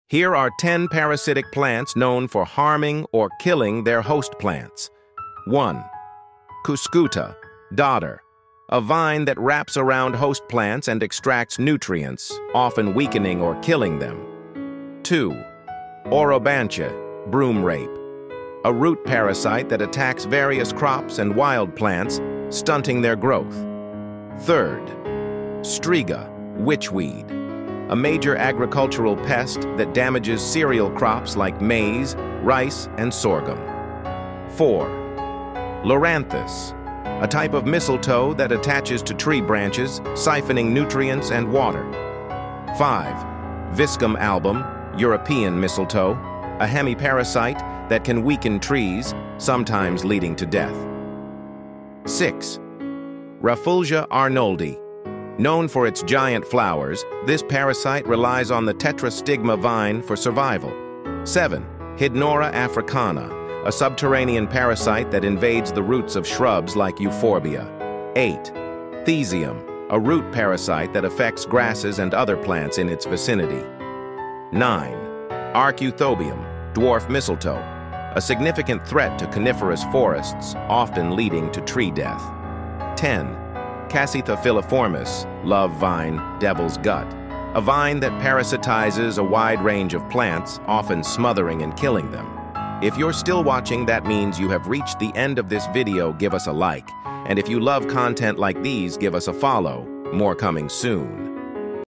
Parasitic plants 🪴
"Explore the eerie world of parasitic plants! From the creeping Cuscuta to the monstrous Hydnora africana, discover how these unique plants survive by feeding off others. Witness nature's dark side with stunning visuals of these fascinating yet deadly species!"